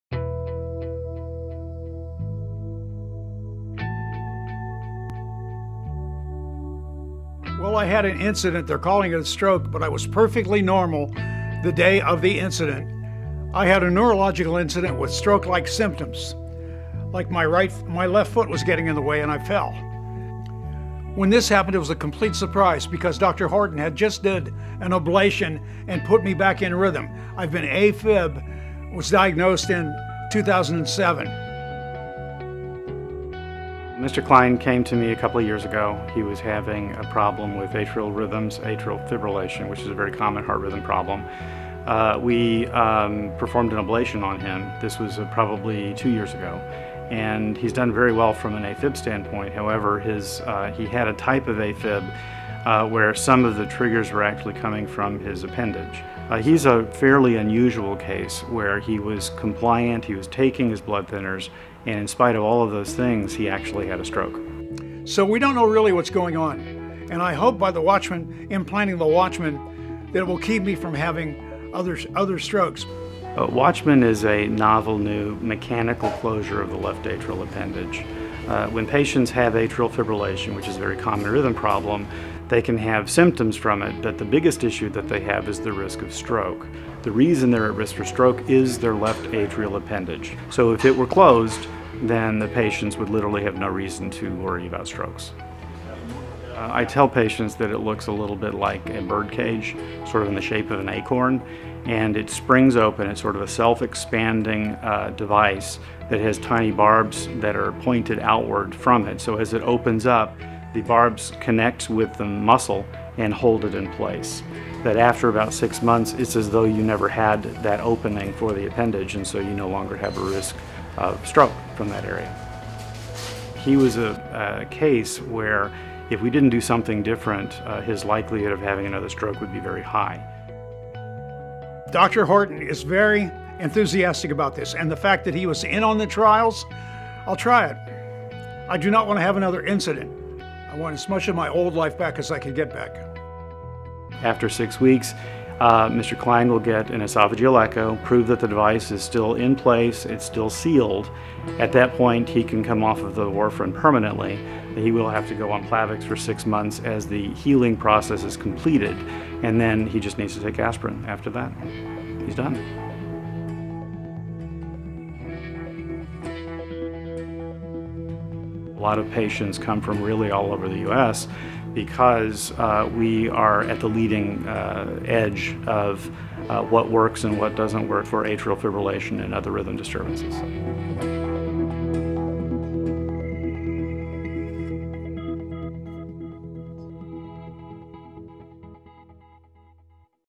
TCAI: 1st Facility in TX to implant the WATCHMAN Left Atrial Appendage Closure Device
Texas Cardiac Arrhythmia Institute (TCAI) at St. David’s Medical Center recently became the first facility in Texas to implant the newly FDA-approved WATCHMAN Left Atrial Appendage Closure (LAAC) Device in a patient with non-valvular Atrial Fibrillation (A Fib).